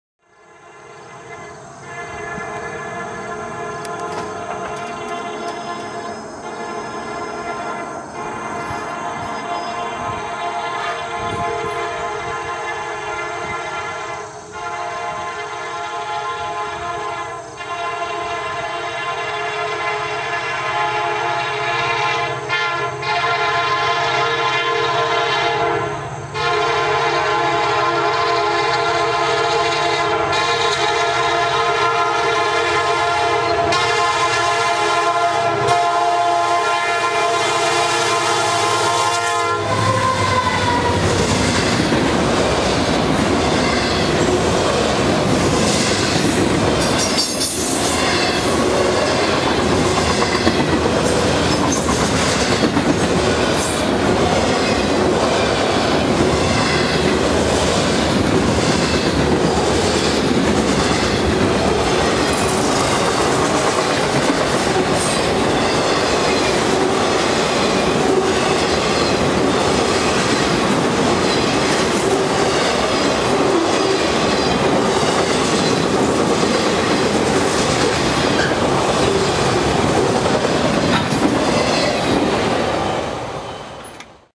Norfolk Southern I90 - Topton, PA
Norfolk Southern I90, the extra for 290, rushes over Haas Street, with a single leader, SD70M-2 #2233 leading.